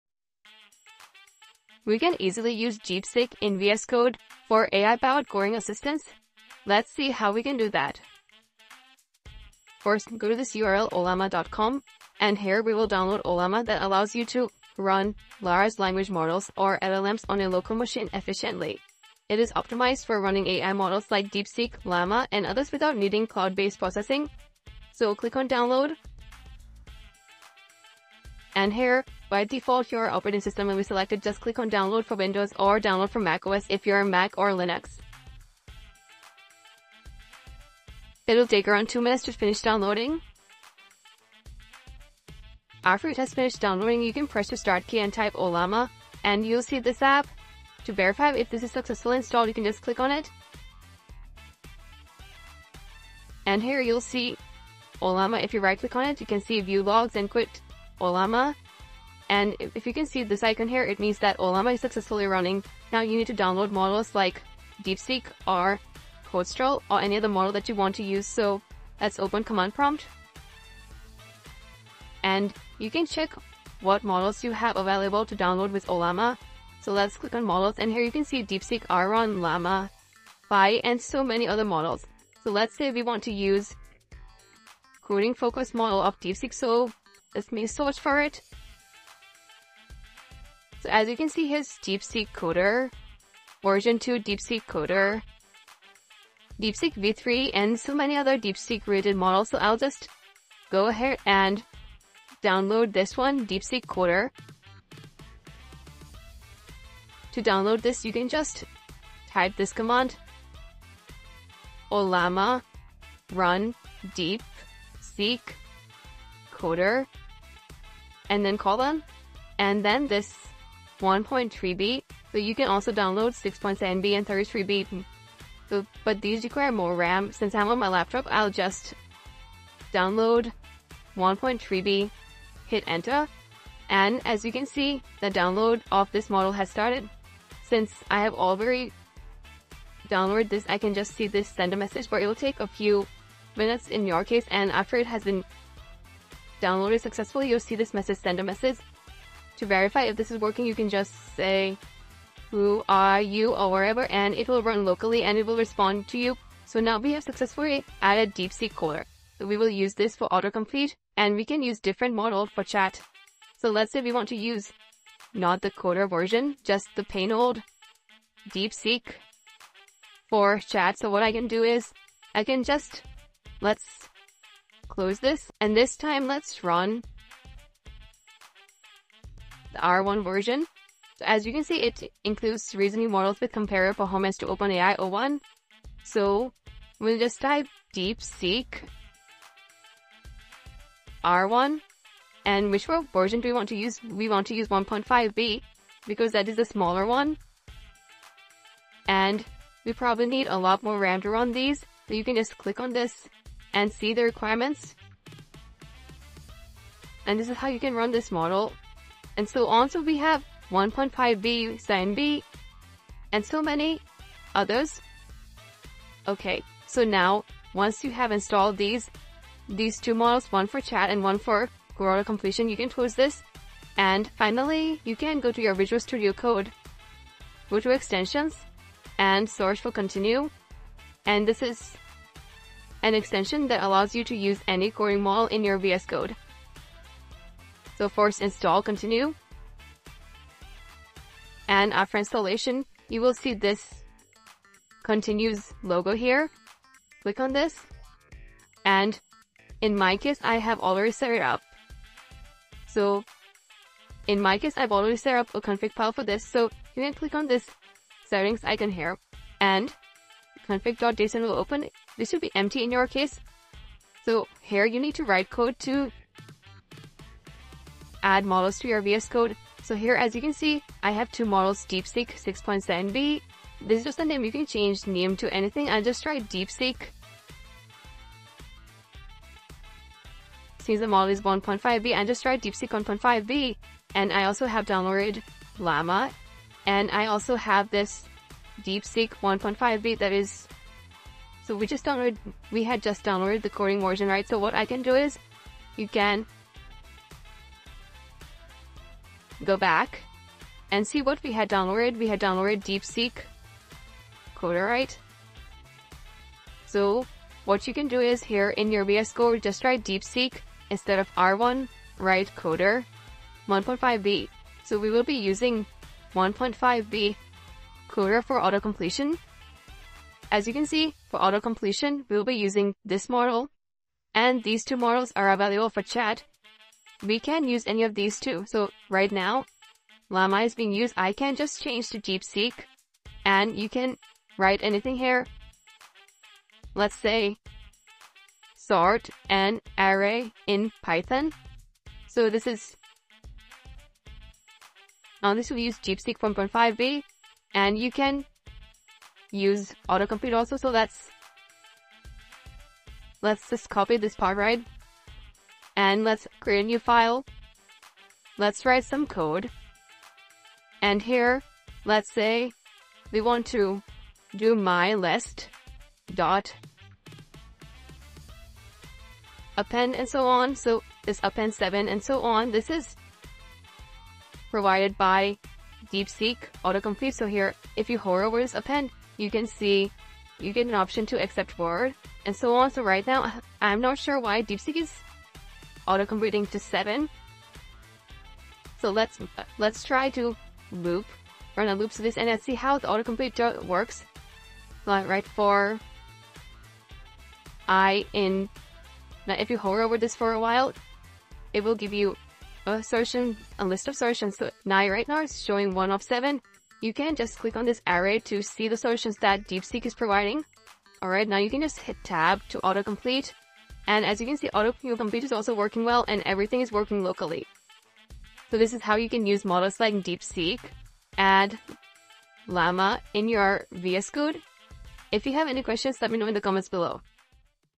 How to Run Deepseek Locally In Visual Studio Code - FREE (2025)
If you want to run Deepseek Chat locally inside Visual Studio Code, this video is for you. In this step-by-step guide, I'll show you how to set up Deepseek, a powerful open-source AI chatbot, directly in VS Code—completely free! 💡

✅ What You'll Learn:
🔹 How to install and configure Deepseek in VS Code
🔹 Running Deepseek locally without an internet connection
🔹 Using Deepseek AI for coding, chat, and assistance
🔹 Best practices for seamless AI integration in VS Code

📌 Why Use Deepseek?
Deepseek is an advanced open-source AI chatbot alternative to ChatGPT that runs offline, giving you privacy, security, and faster response times. Perfect for developers and AI enthusiasts!